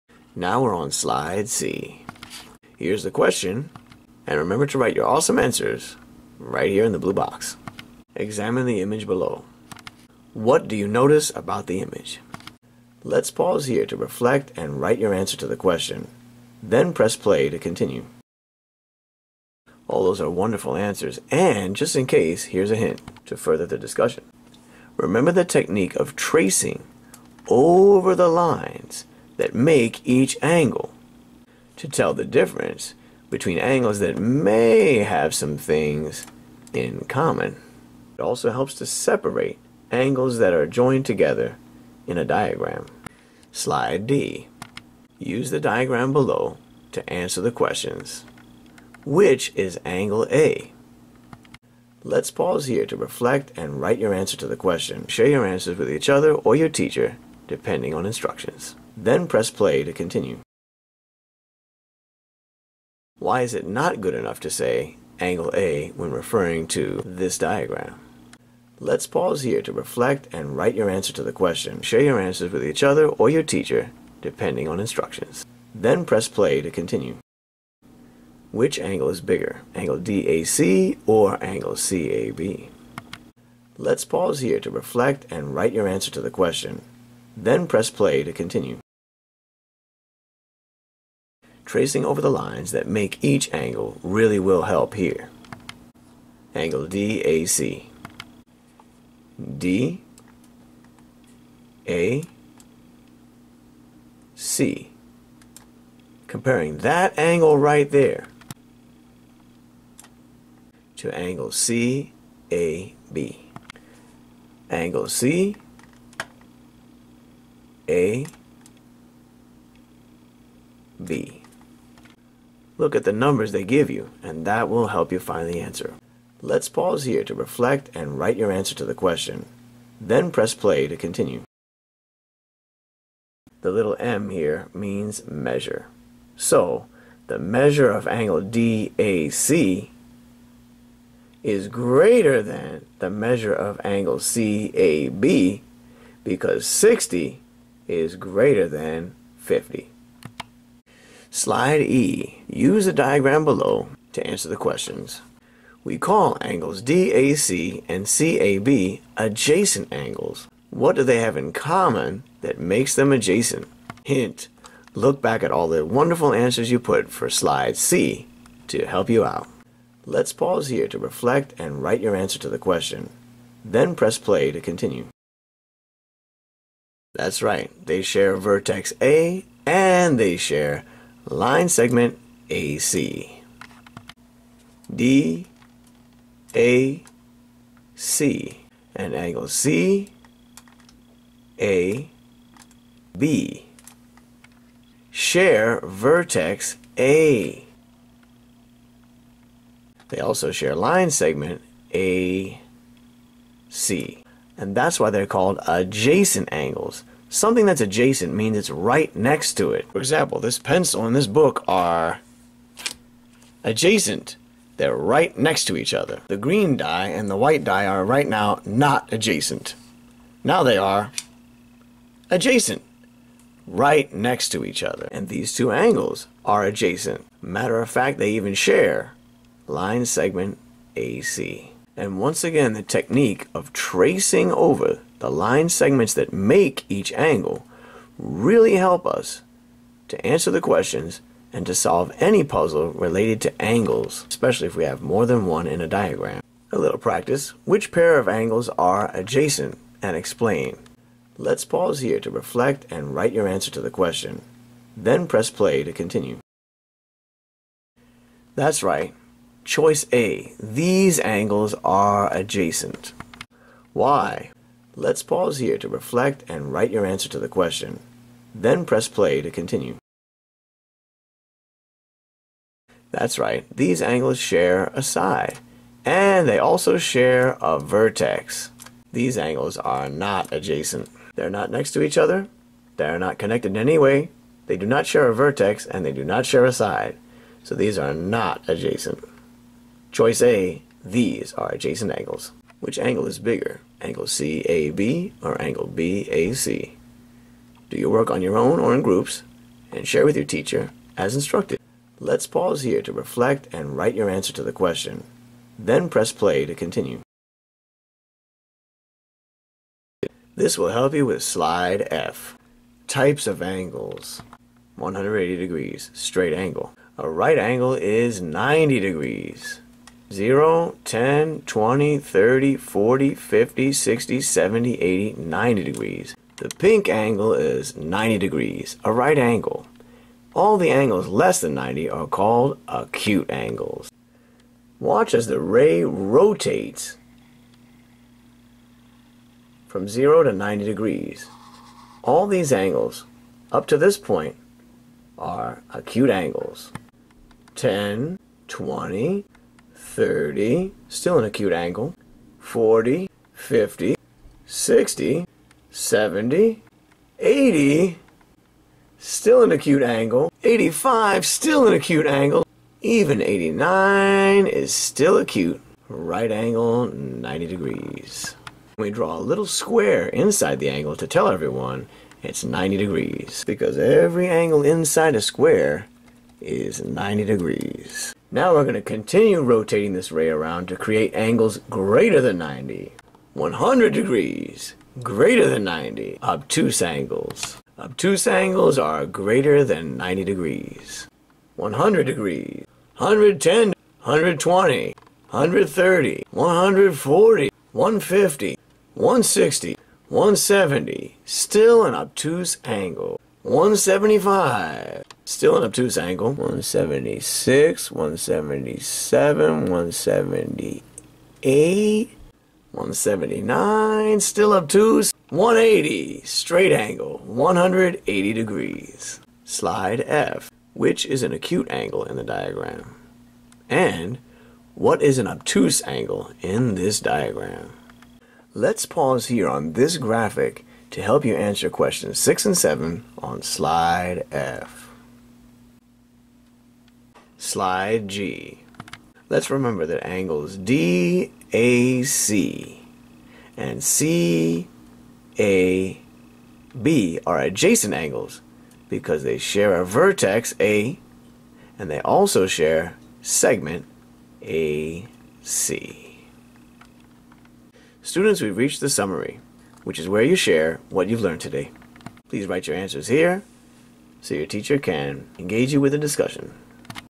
3.1_Math_Grade7_Unit7_AnglesTrianglesandPrisms_Day1 Video 3 of 3 Slides C G new
This is Video 3 out of a 3 video series I created for you, that literally guides students through all of 3.1_Math_Grade7_Unit7_AnglesTrianglesandPrisms_Day1.  It provides scaffolds (direct instruction, vocabulary, strategies and support) for students without giving away the answers.  This way students can engage in the open investigations on their own (asynchronously or flipped classroom) or with the teacher either virtually or in person.

There are pauses during the videos for students to take time to answer questions on their own or in groups or whole class.  Enjoy!

Sincerely,
Jeremy Blynn (Virtual Content Specialist)